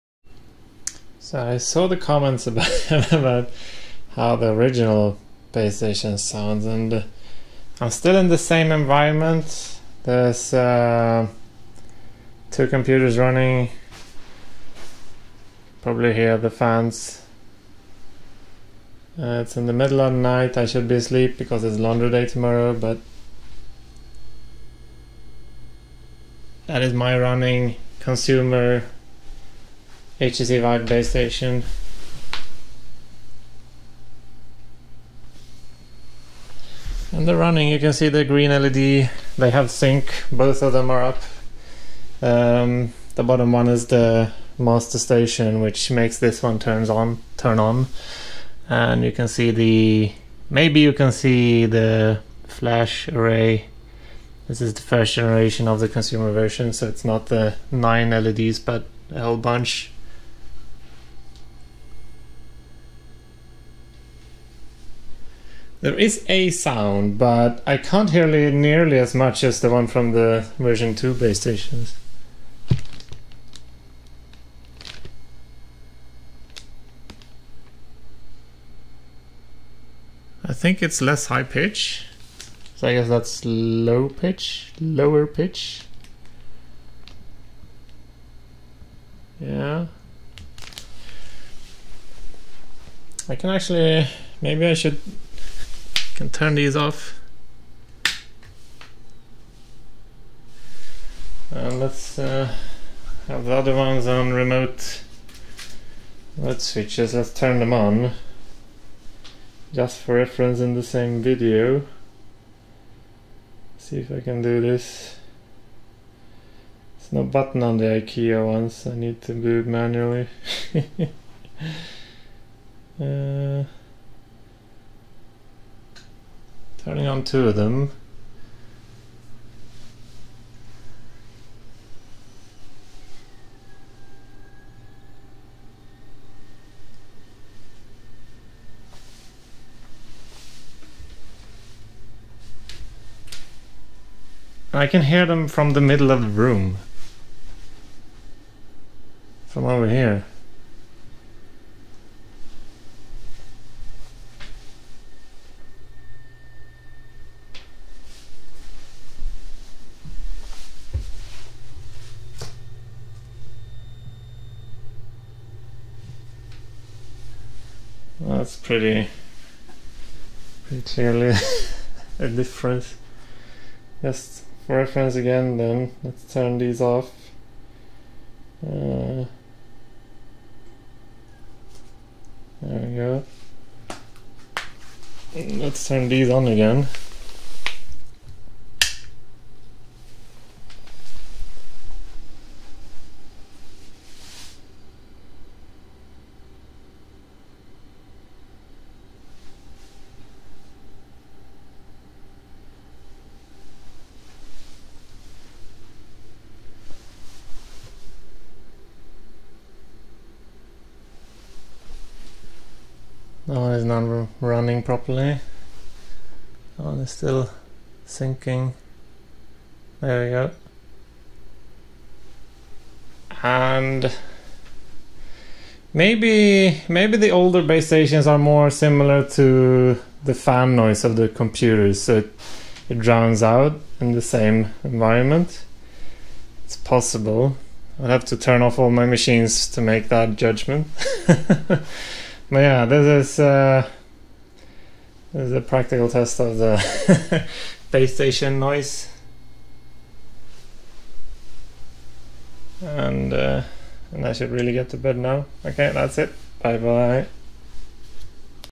Valve Index vs HTC Vive base station noise (binaural, use headphones)
Recorded back in July 2019 when I got my Index, but now with the new wave of new Index users (July 2020) there are a lot of comments regarding the noise the stations make, so I'm making this public to be searchable so there is some kind of reference for this.